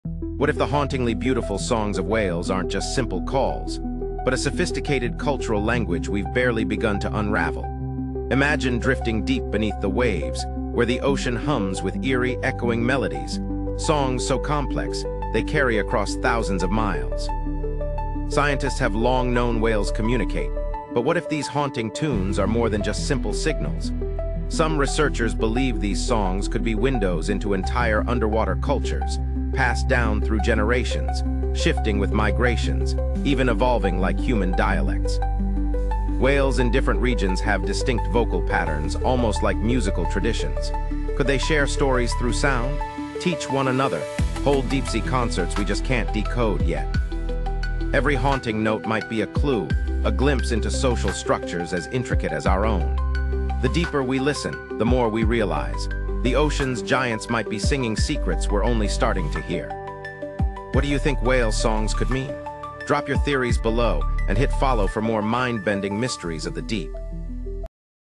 The Hidden Conversations of Whales: What Their Songs Really Mean
Dive deep into the mysterious language of whales—how they communicate across oceans and why their songs are so complex.